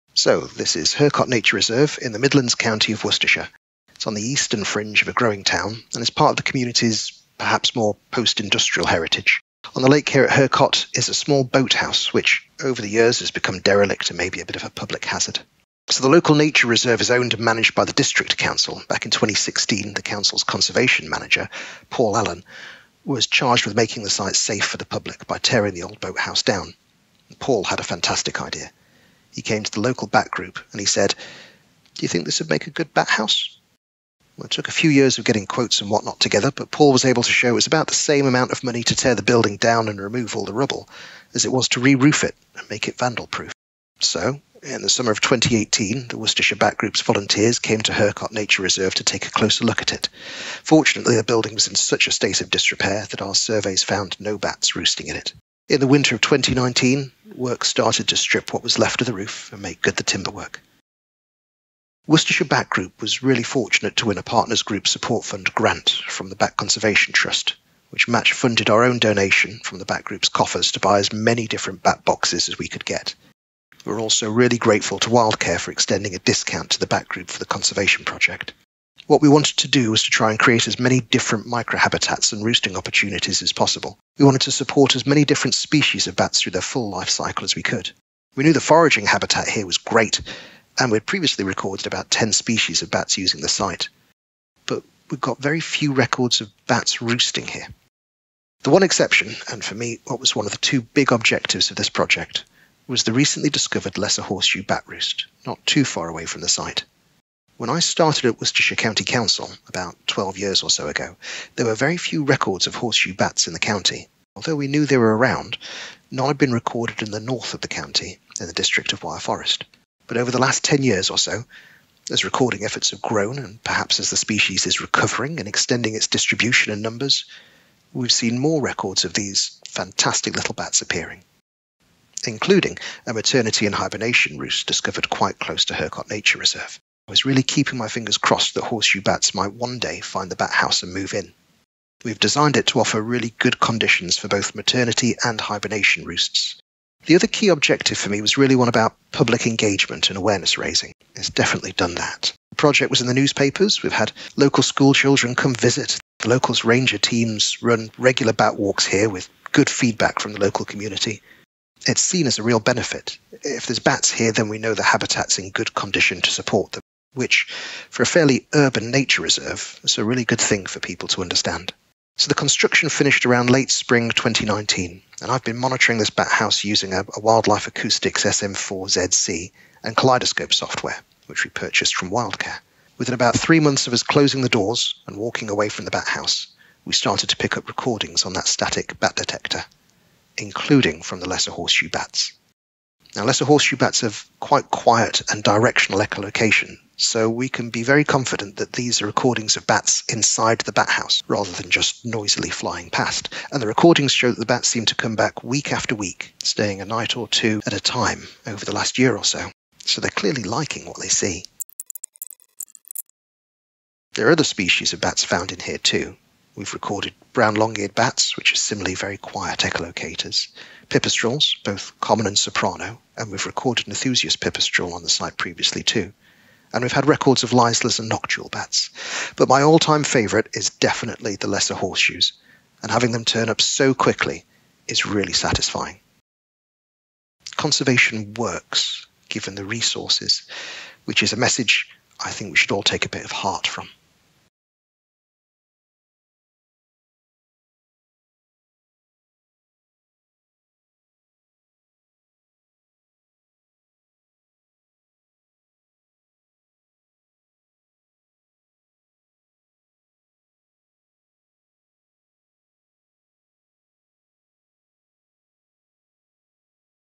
Hurcott Nature Reserve Bat House Project
This video narrated by Cody Levine of Worcestershire County Council takes you on a journey of success. With a lot of hard work, an old boathouse was turned into a bat house and shows that conservation efforts really do work.